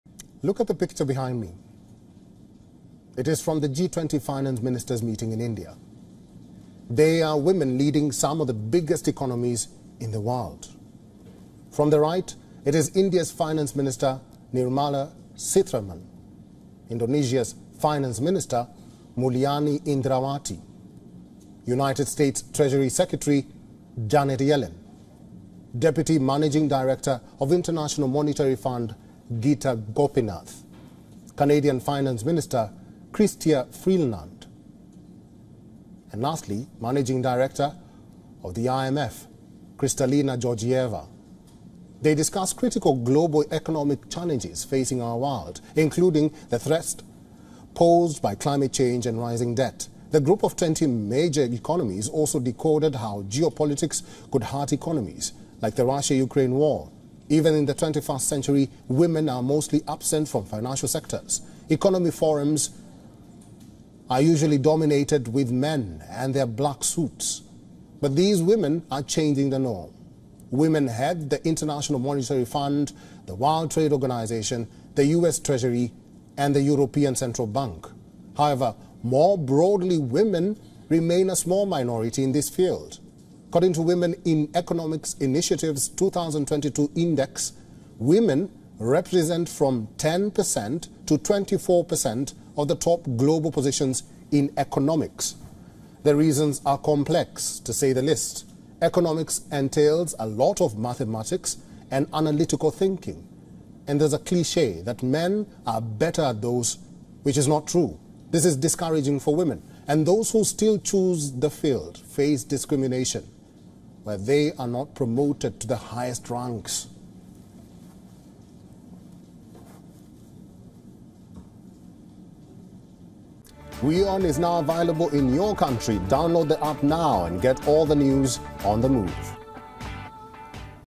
Finance Ministers of India, Indonesia and Canada stand hand in hand | WION Fineprint
The finance ministers of nations including India, Indonesia, Canada, and the US recently discussed pressing global economic issues facing our planet, such as the threat posed by climate change and rising debt. Women are managing some of the biggest economies in the world.